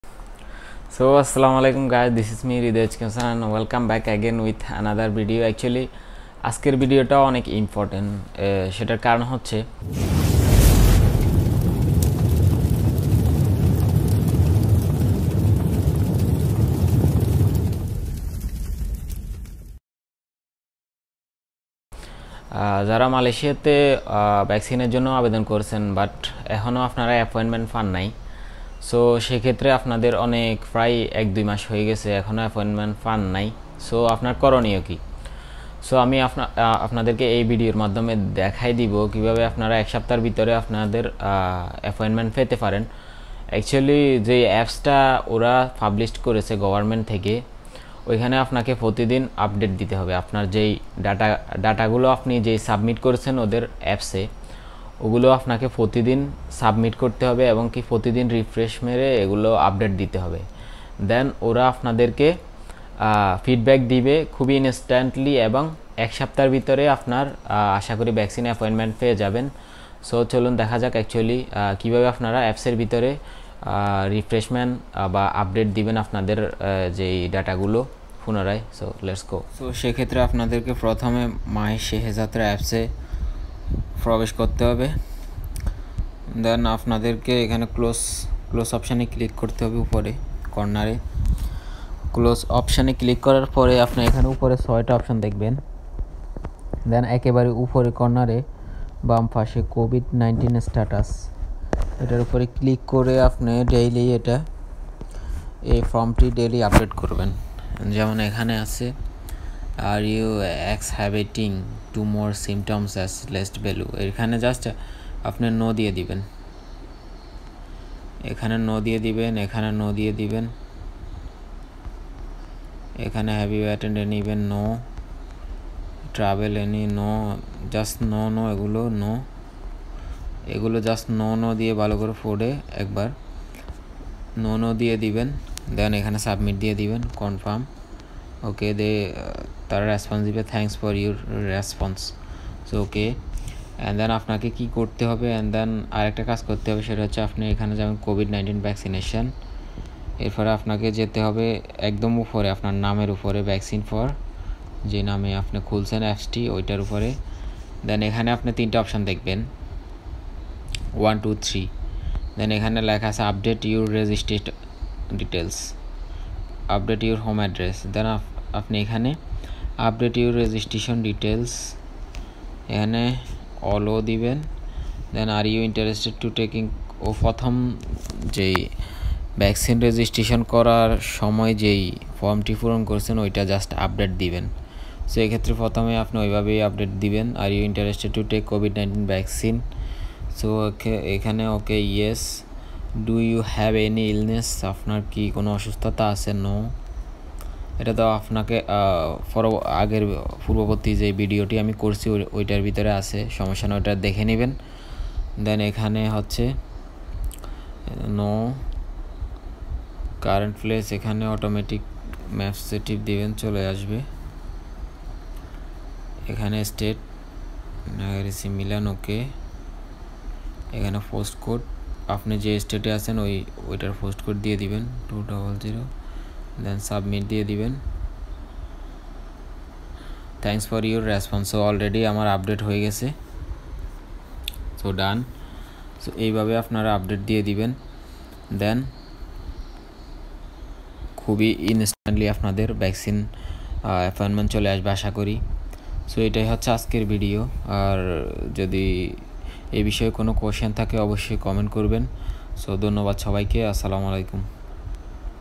How To Update Your MySejahtera Apps |কি ভাবে আপডেট করবেন ভ্যাক্সিনের জন্য Appointment | Tech|Hossain
কি ভাবে ভ্যাক্সিনের জন্য আপনার এপ্লিকেশন আপডেট করবেন দেখুন ভিডিওতে , ভিডিওটি ফলো করে আপডেট করে নিন , আপনার ভ্যাক্সিন অ্যাপয়েন্টমেন্ট খুব তারাতারি চলে আসতে পারে , যাদের অনেক দিন হয়ে গেছে এখনো অ্যাপয়েন্টমেন্ট আসেনি .
in this videos i going to show you guys how to update your mysejahtera apps for appointment